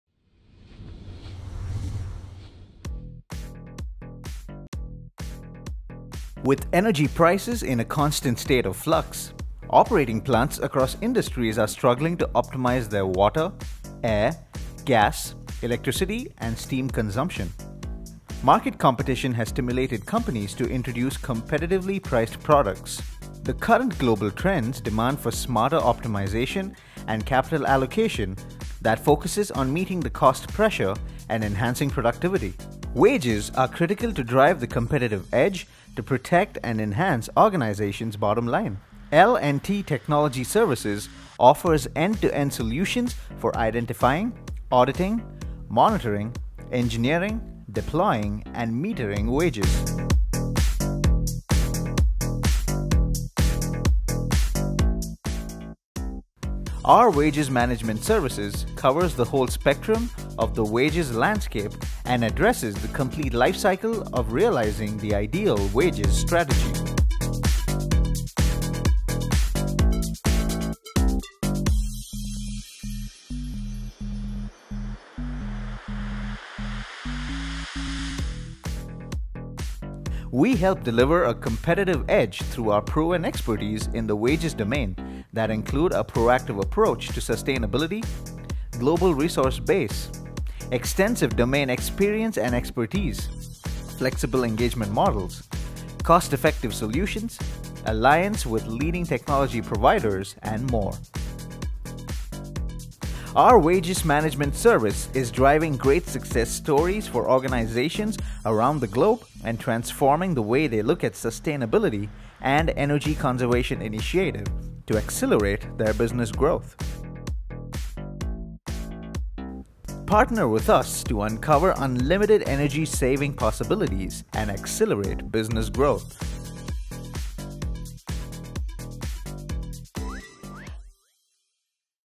Streamline energy management with LTTS Plant Utility Monitoring Solution
Water, air, gas, electricity, and steam – in short, energy, is the driving force behind industries across the world. However, over the last decade, energy prices have gone up due to the global economic forces and geopolitical factors.
At L&T Technology Services, we provide end-to-end plant utility monitoring systems and solutions from the design stage to the digitalization stage.